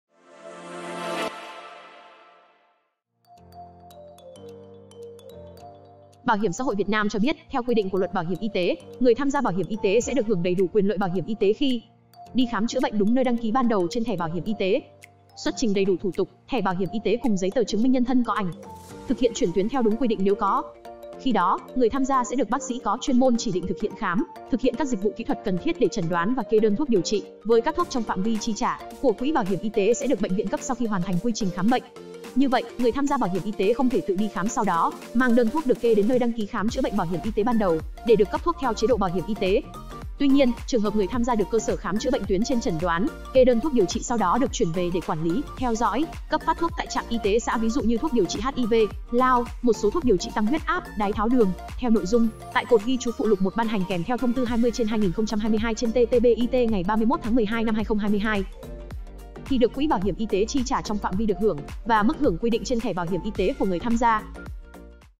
Không đăng ký khám bệnh BHYT ban đầu, có chi trả đơn thuốc?| Báo Lao Động
"Quỹ Bảo hiểm y tế (BHYT) có chi trả cho đơn thuốc khám tại bệnh viện không phải nơi đăng ký khám chữa bệnh BHYT ban đầu không?" là băn khoăn của người dân.
* Đường dây nóng: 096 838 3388